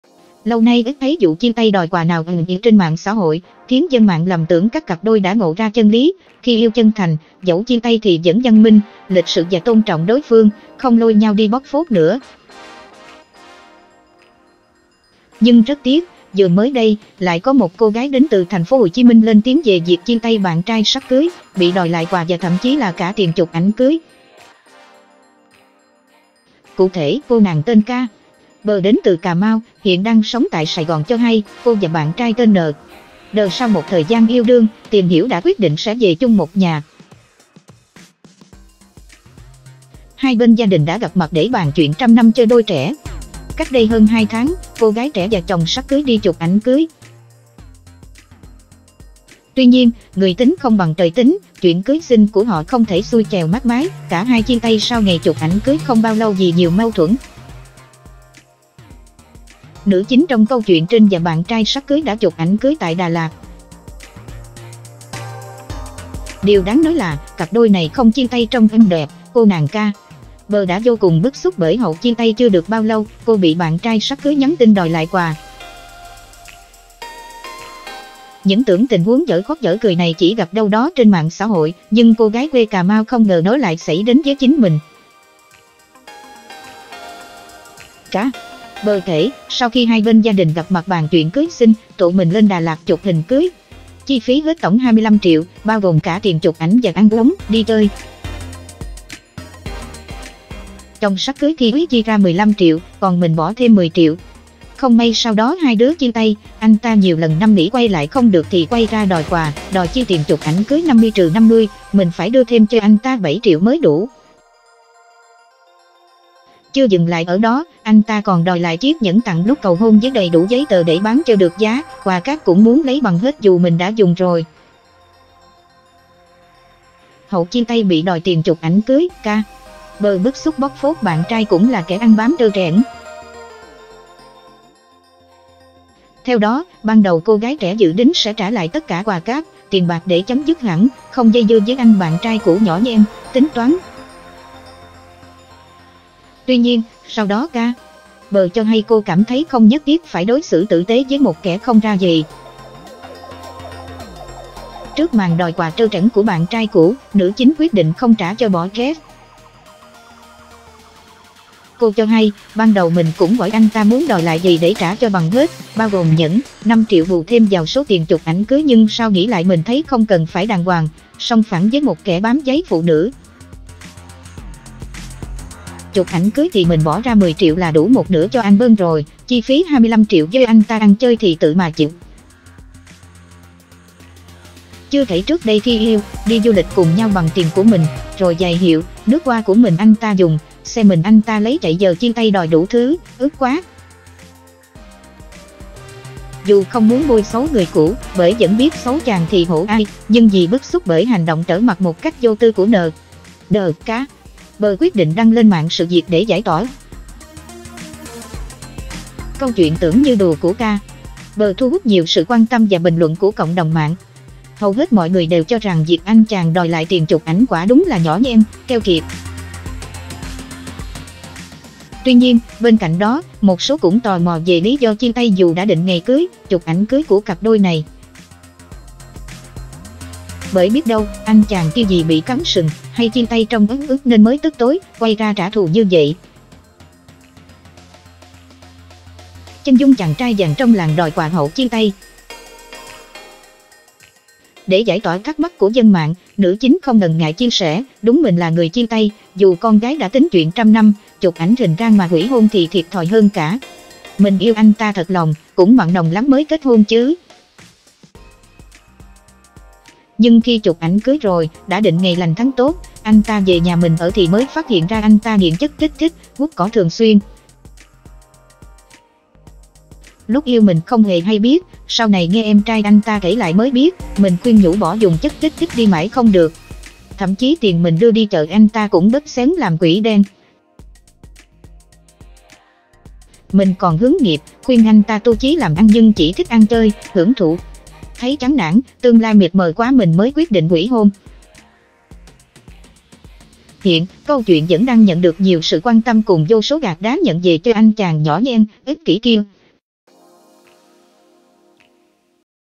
✅ Chụp ảnh cưới xong thì “đường ai nấy đi', cô gái bị bạn trai đòi quà, nhẫn cưới và cả tiền chụp
Đặt Mua  Qua ZALO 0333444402

✅Lâu nay ít thấy vụ chia tay đòi quà nào ầm ĩ trên mạng xã hội, khiến dân mạng lầm tưởng các cặp đôi đã "ngộ" ra chân lý: Khi yêu chân thành, dẫu chia tay thì vẫn văn minh, lịch sự và tôn trọng đối phương, không lôi nhau đi "bóc phốt" nữa.

Nhưng rất tiếc, vừa mới đây, lại có một cô gái đến từ thành phố Hồ Chí Minh lên tiếng về việc chia tay bạn trai sắp cưới, bị đòi lại quà và thậm chí là cả tiền… chụp ảnh cưới.
Cụ thể, cô nàng tên K.P đến từ Cà Mau, hiện đang sống tại Sài Gòn cho hay, cô và bạn trai tên N.Đ sau một thời gian yêu đương, tìm hiểu đã quyết định sẽ về chung một nhà.
Hai bên gia đình đã gặp mặt để bàn chuyện trăm năm cho đôi trẻ. Cách đây hơn 2 tháng, cô gái trẻ và chồng sắp cưới đi chụp ảnh cưới.
Tuy nhiên, "người tính không bằng trời tính", chuyện cưới xin của họ không thể "xuôi chèo mát mái", cả hai chia tay sau ngày chụp ảnh cưới không bao lâu vì nhiều mâu thuẫn.
Nữ chính trong câu chuyện trên và bạn trai sắp cưới đã chụp ảnh cưới tại Đà Lạt
Điều đáng nói là, cặp đôi này không chia tay trong êm đẹp, cô nàng K.P đã vô cùng bức xúc bởi hậu chia tay chưa được bao lâu, cô bị bạn trai sắp cưới nhắn tin đòi lại quà.
Những tưởng tình huống "dở khóc dở cười" này chỉ gặp đâu đó trên mạng xã hội, nhưng cô gái quê Cà Mau không ngờ nó lại xảy đến với chính mình.
K.P kể: "Sau khi hai bên gia đình gặp mặt bàn chuyện cưới xin, tụi mình lên Đà Lạt chụp hình cưới. Chi phí hết tổng 25 triệu, bao gồm cả tiền chụp ảnh và ăn uống, đi chơi…
Chồng sắp cưới khi ấy chi ra 15 triệu, còn mình bỏ thêm 10 triệu. Không may sau đó hai đứa chia tay, anh ta nhiều lần năm nỉ quay lại không được thì quay ra đòi quà, đòi chia tiền chụp ảnh cưới 50-50, mình phải đưa thêm cho anh ta 7 triệu mới đủ.
Chưa dừng lại ở đó, anh ta còn đòi lại chiếc nhẫn tặng lúc cầu hôn với đầy đủ giấy tờ để bán cho được giá, quà cáp cũng muốn lấy bằng hết dù mình đã dùng rồi".
Hậu chia tay bị đòi tiền chụp ảnh cưới, K.P bức xúc "bóc phốt" bạn trai cũng là kẻ ăn bám trơ trẽn.
Theo đó, ban đầu cô gái trẻ dự đính sẽ trả lại tất cả quà cáp, tiền bạc để chấm dứt hẳn, không dây dưa với anh bạn trai cũ nhỏ nhen, tính toán.
Tuy nhiên, sau đó K.P cho hay cô cảm thấy không nhất thiết phải đối xử tử tế với một kẻ không ra gì.
Trước màn đòi quà trơ trẽn của bạn trai cũ, nữ chính quyết định không trả cho "bõ ghét"
Cô cho hay: "Ban đầu mình cũng hỏi anh ta muốn đòi lại gì để trả cho bằng hết, bao gồm nhẫn, 5 triệu bù thêm vào số tiền chụp ảnh cưới… Nhưng sau nghĩ lại mình thấy không cần phải đàng hoàng, song phẳng với một kẻ bám váy phụ nữ.
Chụp ảnh cưới thì mình bỏ ra 10 triệu là đủ một nửa cho album rồi, chi phí 25 triệu do anh ta ăn chơi thì tự mà chịu.
Chưa kể trước đây khi yêu, đi du lịch cùng nhau bằng tiền của mình, rồi giày hiệu, nước hoa của mình anh ta dùng, xe mình anh ta lấy chạy…Giờ chia tay đòi đủ thứ, ức quá".
Dù không muốn bôi xấu người cũ, bởi vẫn biết "xấu chàng thì hổ ai", nhưng vì bức xúc bởi hành động trở mặt một cách vô tư của N.Đ, K.P quyết địn